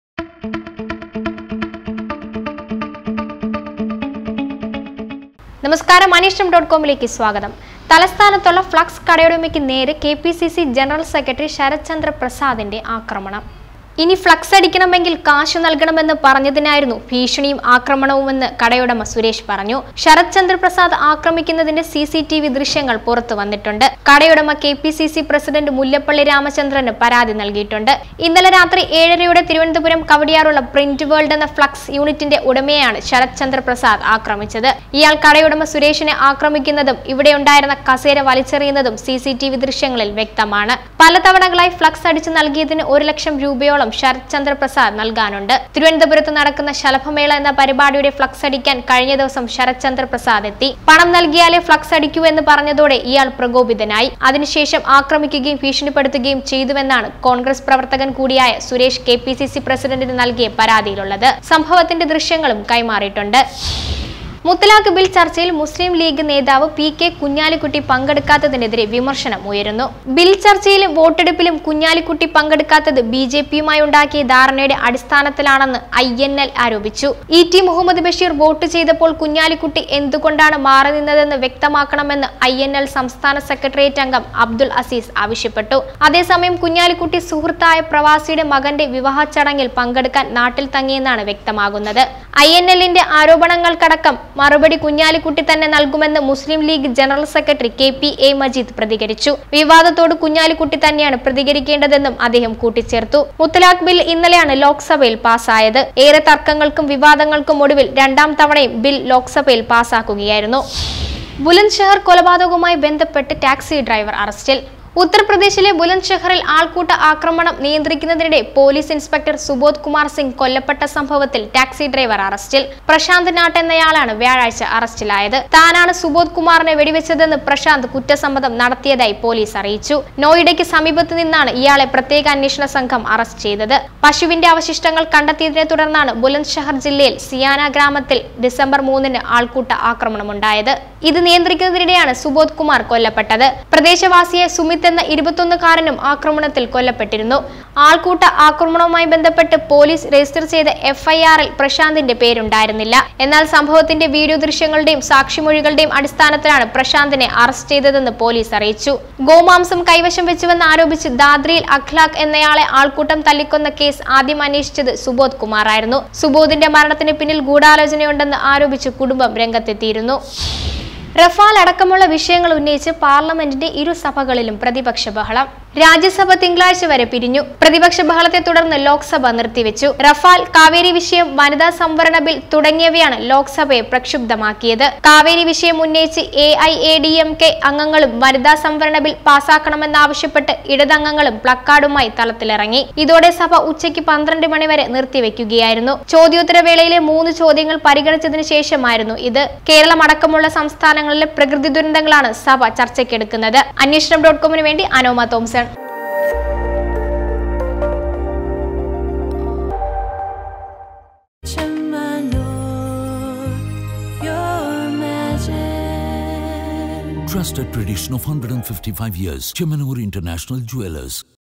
congress leader sharath chandra prasad attacked flux shop owner
കടയുടമയെ കോണ്‍ഗ്രസ് നേതാവ് ശരത് ചന്ദ്ര പ്രസാദ് ആക്രമിച്ചതായി പരാതി

മുത്തലാഖ് ചർച്ചയിൽ പങ്കെടുക്കാതെ കുഞ്ഞാലിക്കുട്ടി; വിവാദം കത്തുന്നു

ബുലന്ദ്ശഹർ കൊലപാതകം: ടാക്‌സി ഡ്രൈവര്‍ അറസ്റ്റില്‍

പാര്‍ലമെന്റിന്റെ ഇരുസഭകളിലും ബഹളം; രാജ്യസഭ തിങ്കളാഴ്ച വരെ പിരിഞ്ഞു